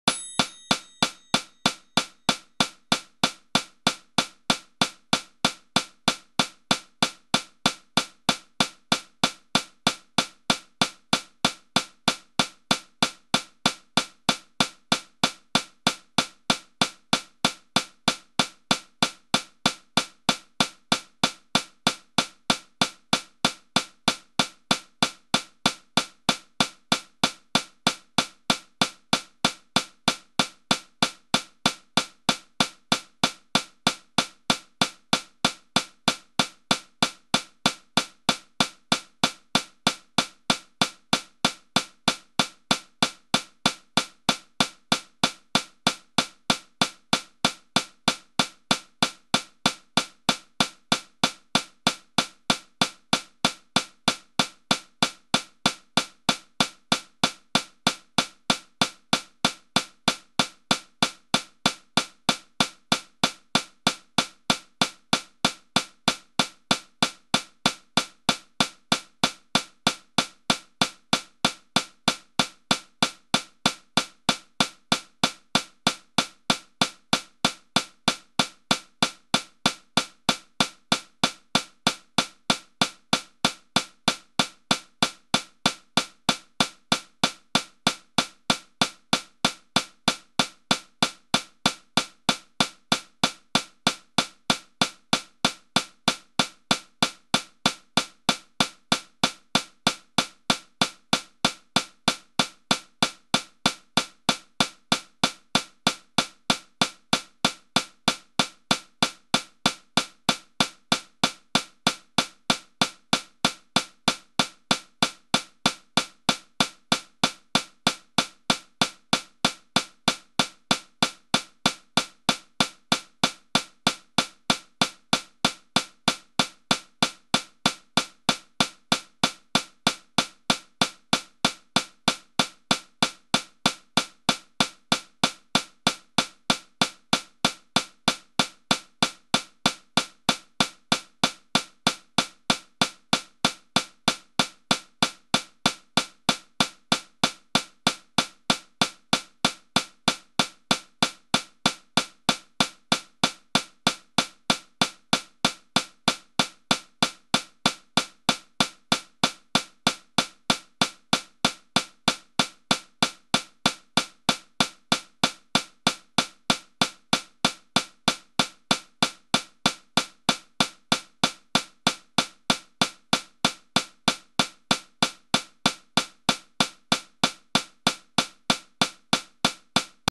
190 BPM Online Metronome (3 min) | StudyBass
Try out the online metronome at StudyBass!
Quickly and easily adjust the tempo, loop tempos, and auto-increment the metronome speed at specific time intervals with the advanced online metronome.